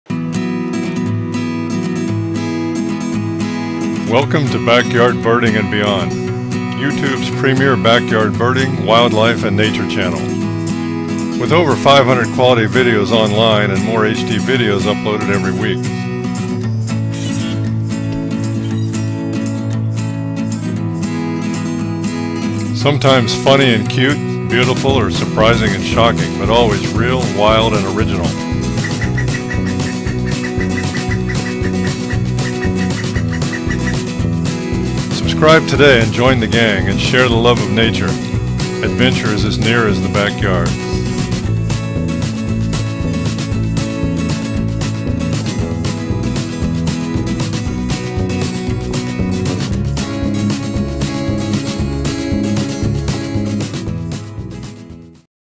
Backyard Birding and Beyond Channel Trailer
Welcome to Backyard Birding .and Beyond! Sharing original videos of birds, wildlife and Nature observed in the Backyard and in my travels beyond. New HD videos are uploaded weekly. Adventure is as near as the Backyard! Backyard Birder (BB)
Video clips included:
White Breasted Nuthatch, Eastern Screech Owl, Painted Bunting, Pileated Woodpecker, Baby Red Squirrels